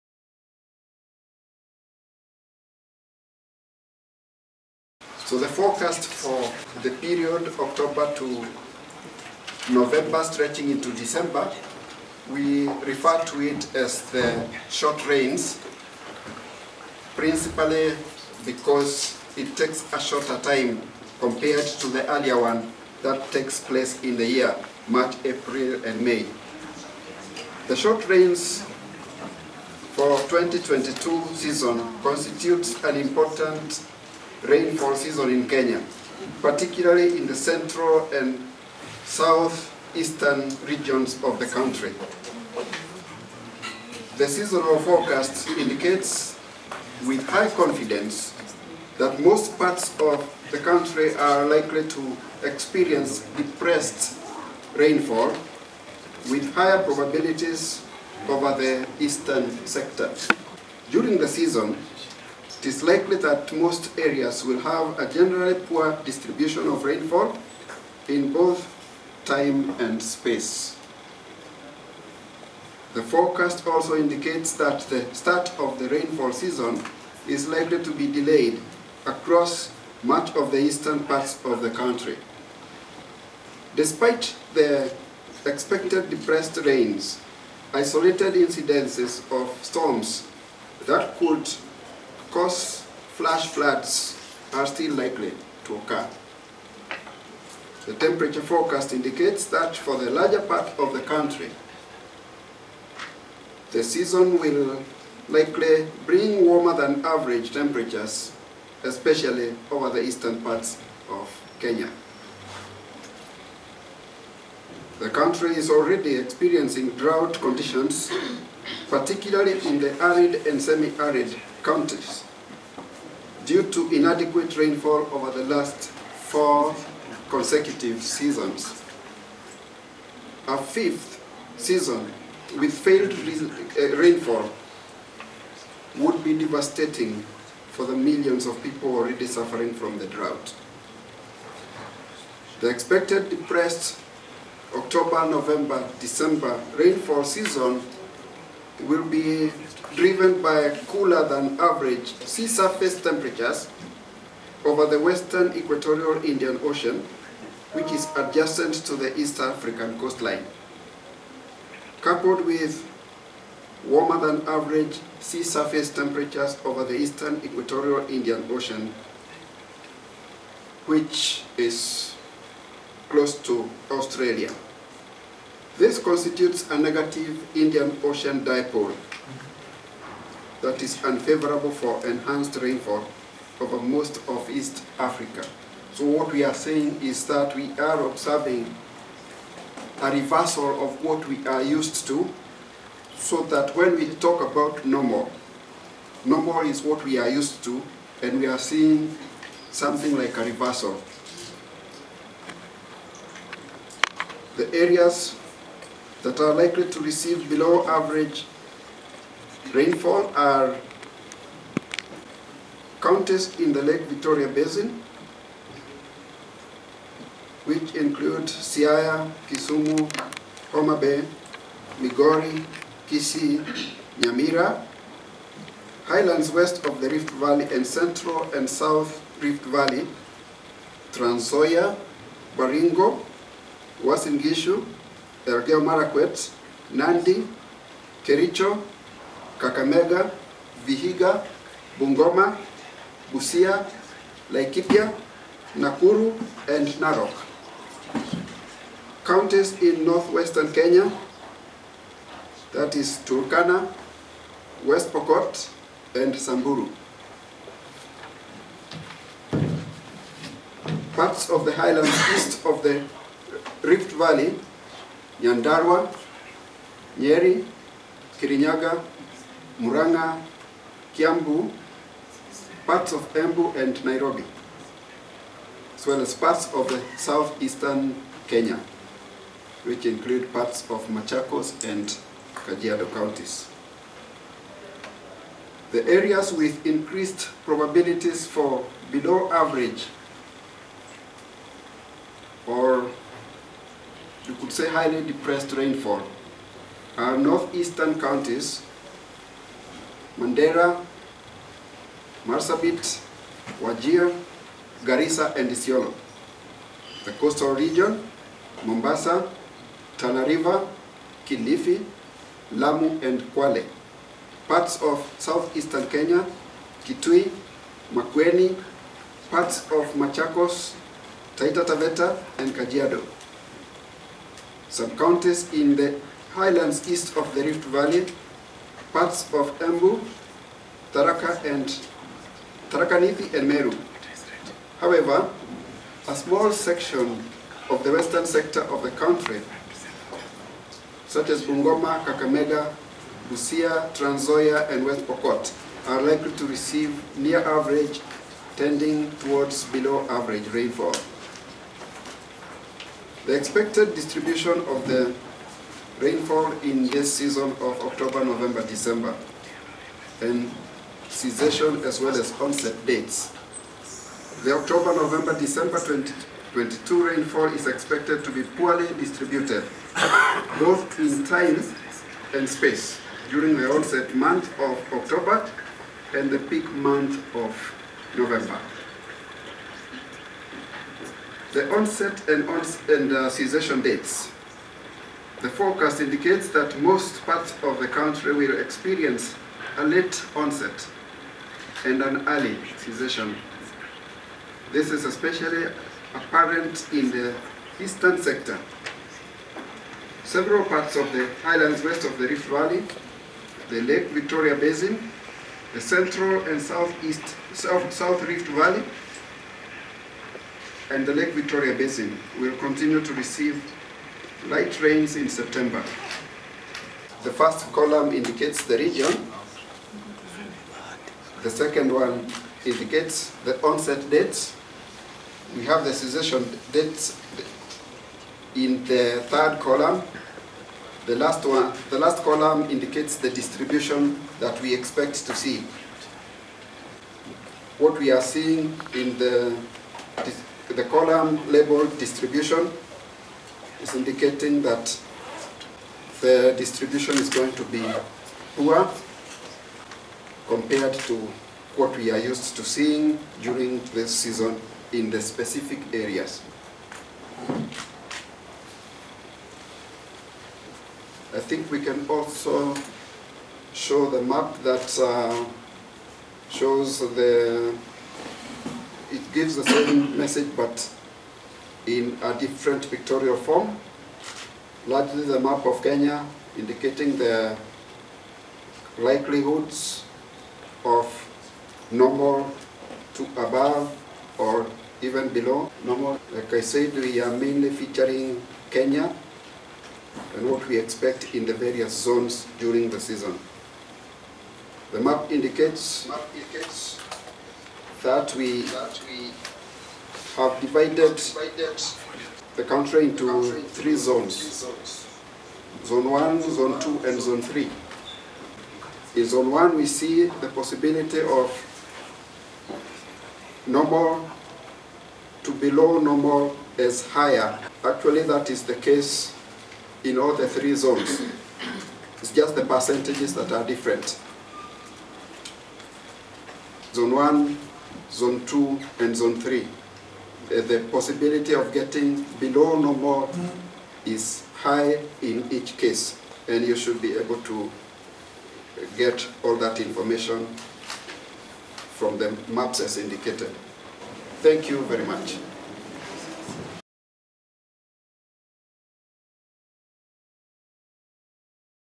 Short Rains Seasonal forecast OND 2022
This is a clip of the Short rains seasonal forecast Otober, November, December 2022 in Kenya. Giving an over view of the expected rainfall during the season to enable planning and mitigation for the Country. Was released by Dr. David Gikungu the director Kenya Meteorological Department on 2nd September 2022.